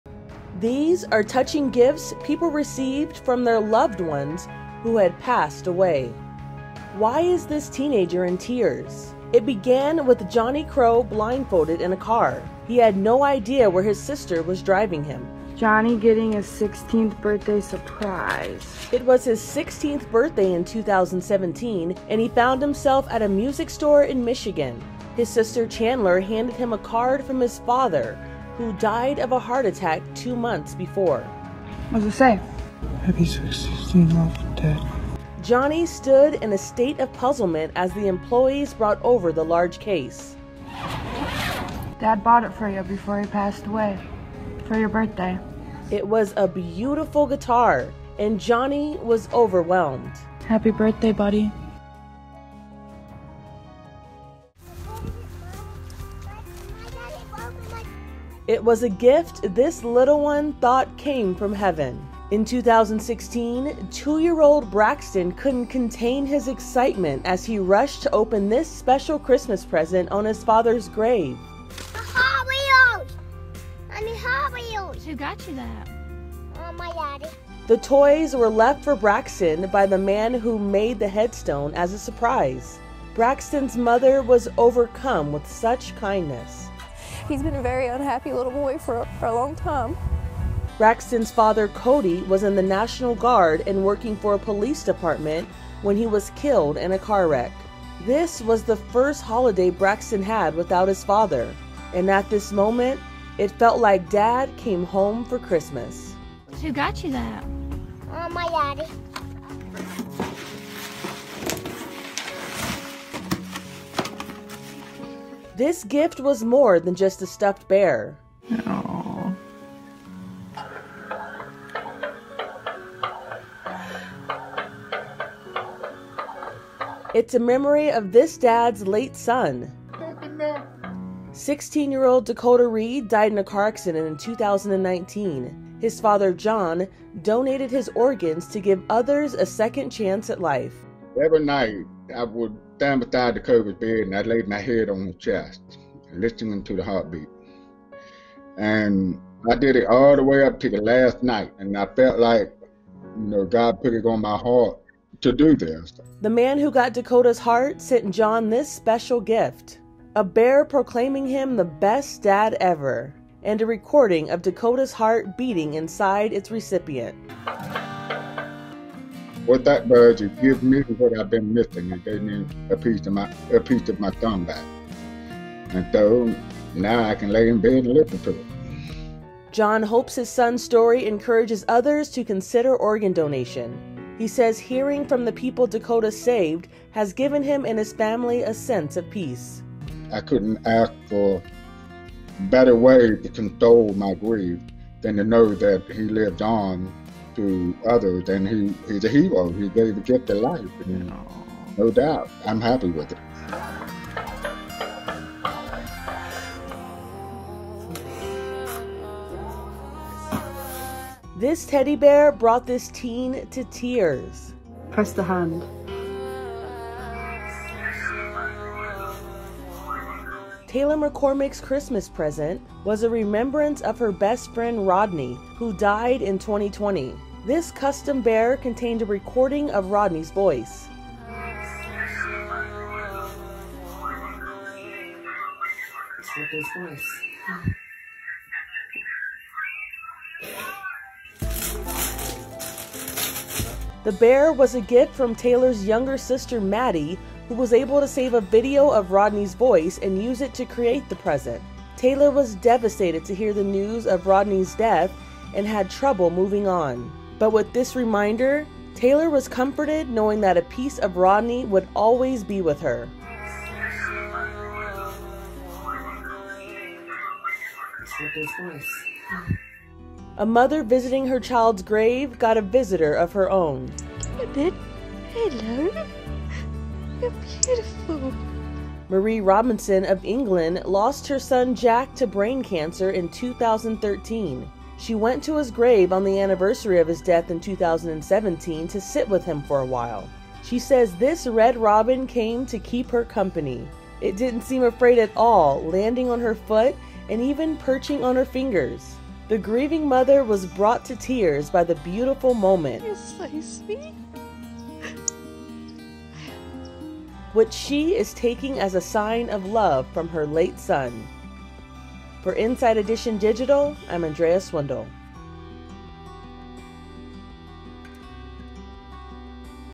These Gifts Are From Beyond the Grave
These are touching gifts people received after their loved ones passed away. In 2017, Johnny Crow discovered his dad had set up a birthday surprise before passing away. In 2016, 2-year-old Braxton got a pleasant surprise when he visited his dad’s tombstone.  In 2022, Taylor McCormack’s Christmas present from her sister was a remembrance of her best friend, Rodney, who died two years before. Inside Edition Digital’s Andrea Swindall has more.